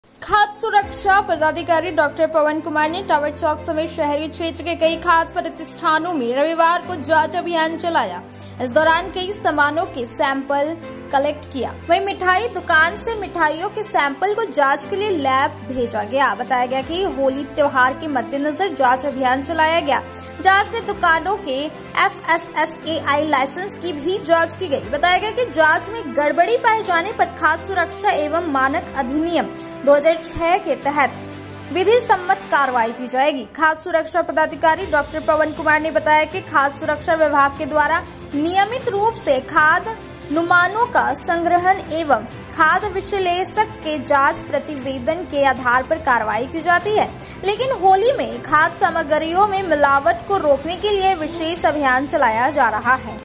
खाद्य सुरक्षा पदाधिकारी डॉ. पवन कुमार ने टावर चोक समेत कई खाद्य प्रतिष्ठानों में जांच अभियान चलाया।
खाद्य सुरक्षा पदाधिकारी डॉ. पवन कुमार ने टावर चोक समेत शहरी क्षेत्र के कई खाद्य प्रतिष्ठानों में रविवार को जांच अभियान चलाया।Newslinegiridih